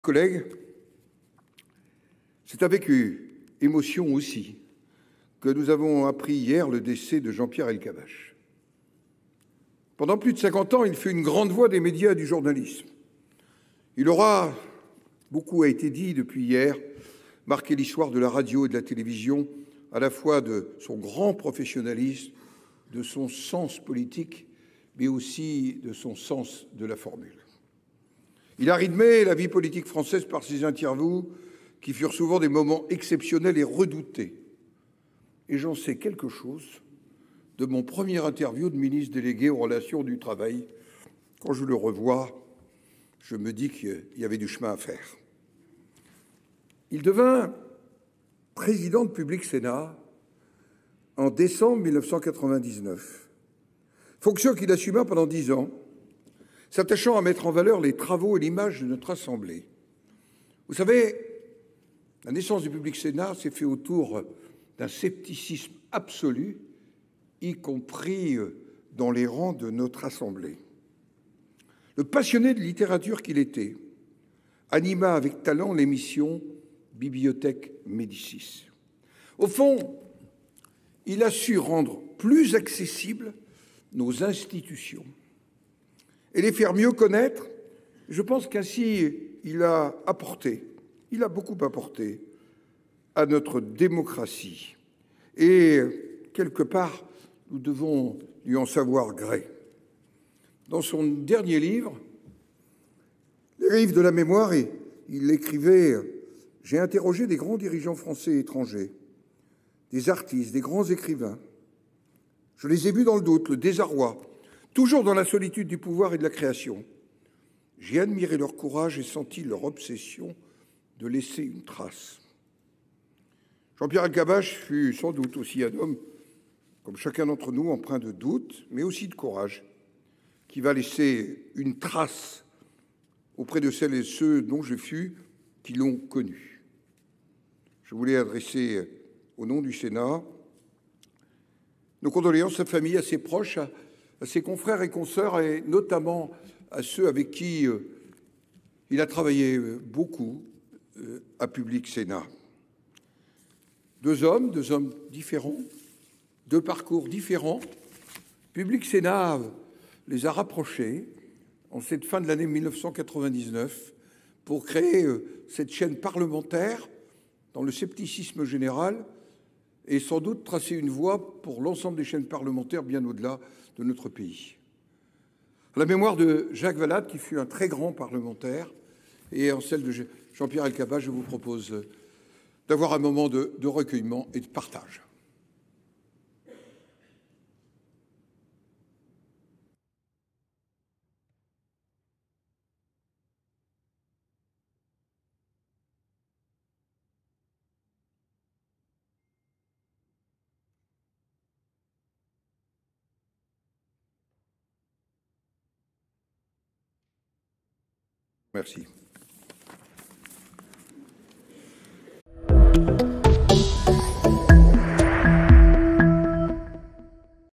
L'hommage du Sénat à Jean-Pierre Elkabbach
Ce 4 octobre à l'ouverture de la séance publique, Gérard Larcher le Président du Sénat a rendu hommage au journaliste décédé hier qui fût aussi le PDG de Public Sénat